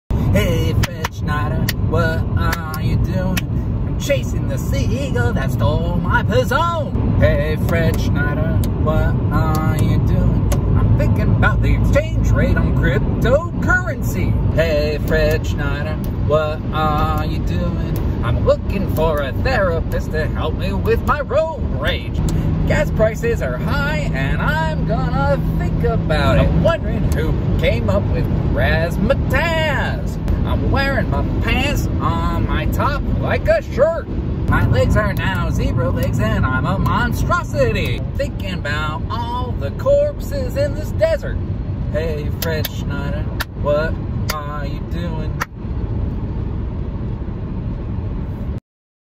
Hey Fred Schneider, what are you doing?
Playing an improv game to myself entertained on a road trip